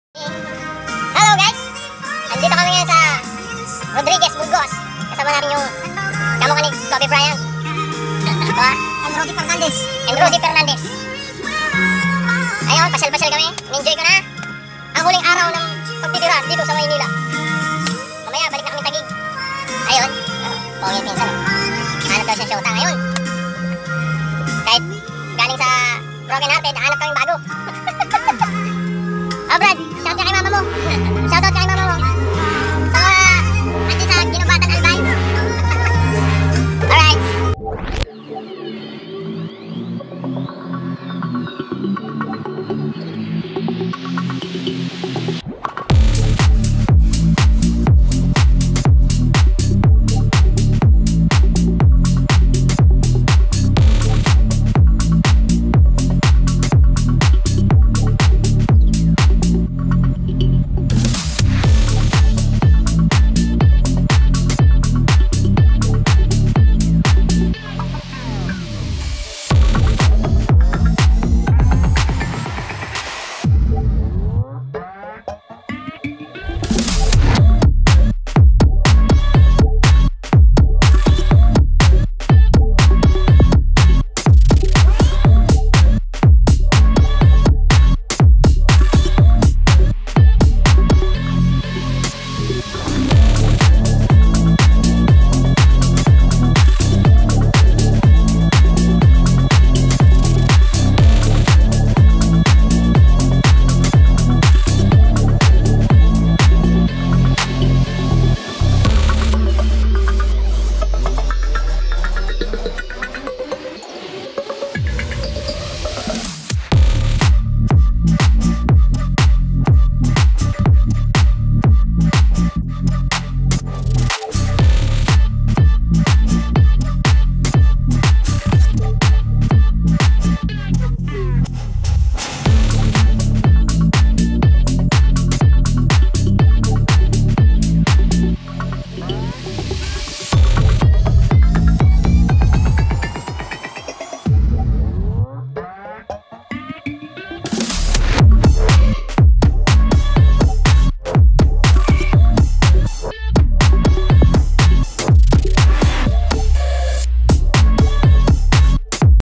Monday Adventure at Mt. Montalban
Enjoying ridetrip with my Cousin Leevan Paraiso and Co worker Albert Ocbian to Mt. Montalban View and Wawadam Sight.